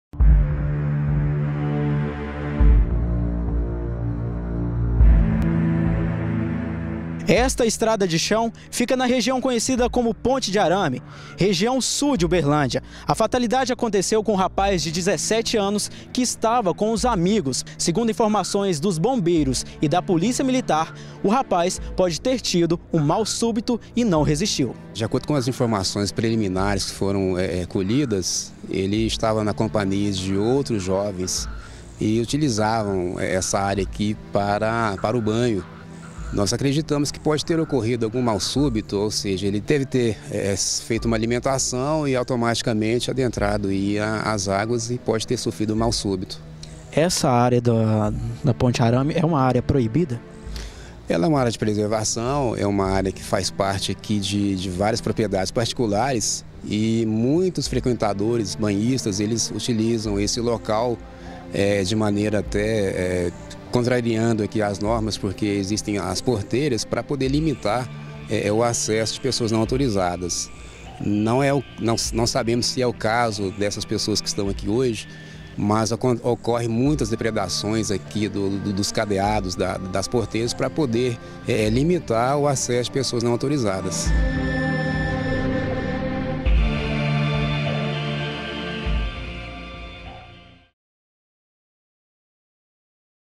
Adolescente morre afogado na região conhecida como Ponte de Arame
Reportagem de Júnior Caritel com imagens de Cláudio Costa para No Foco da Notícia de 13/07/15